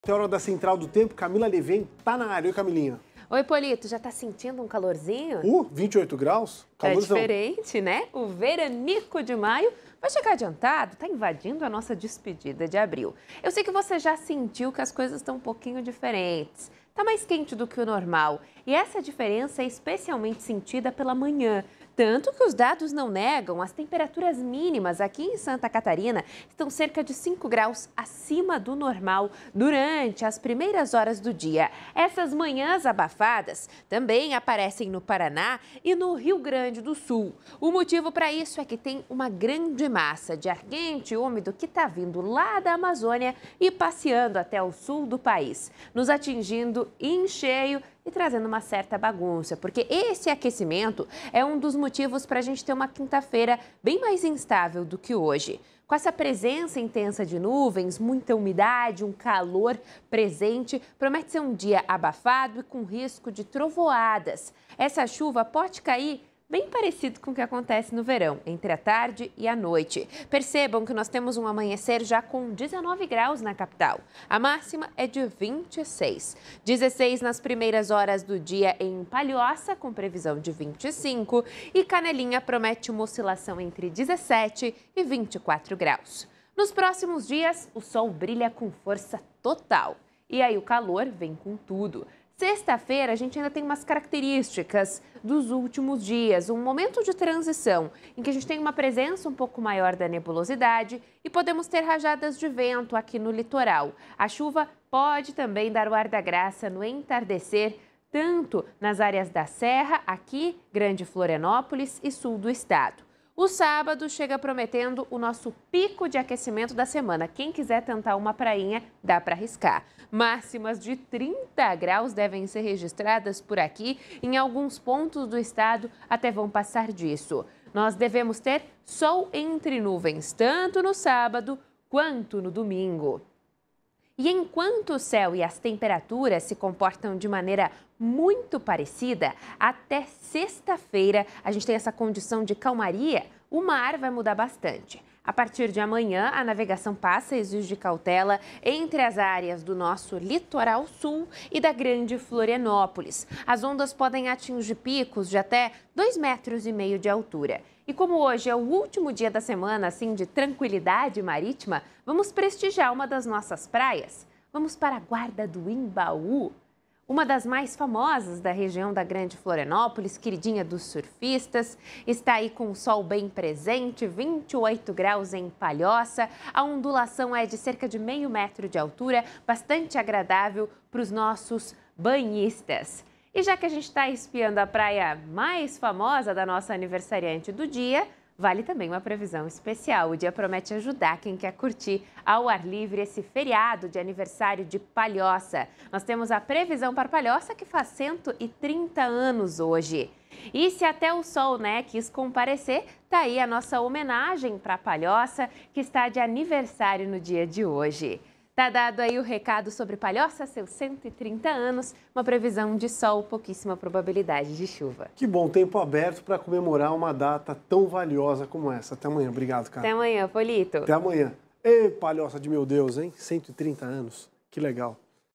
Central do Tempo - Sol brilhante e temperaturas altas nos próximos dias
Nos próximos dias, prepare-se para o sol brilhar com força e o calor vir com tudo em Santa Catarina. A sexta-feira ainda trará algumas características dos últimos dias, com momentos de transição, rajadas de vento no litoral e chuva na serra e sul do estado. Porém, o sábado promete ser o pico de aquecimento da semana, com temperaturas chegando à casa dos 30 graus e até mesmo ultrapassando esse limite, acompanhadas de sol entre nuvens. Confira todos os detalhes sobre a previsão do tempo para esta quarta-feira (24) com a Camila Levien.